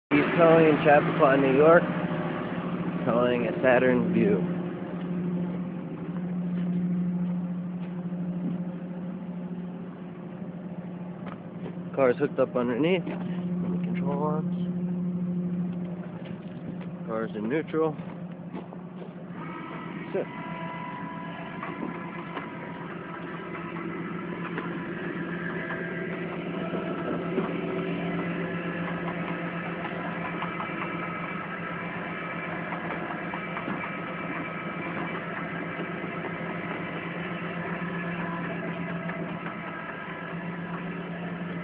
Towing company Chappauqa NY Towing Saturn
East Towing towing a Saturn from Chappaqua NY to Vail Buick in Bedford Hills NY.  We offer towing towing in Chappaqua NY and Bedford Hills NY to all dealerships.  We also provide towing service for I-684.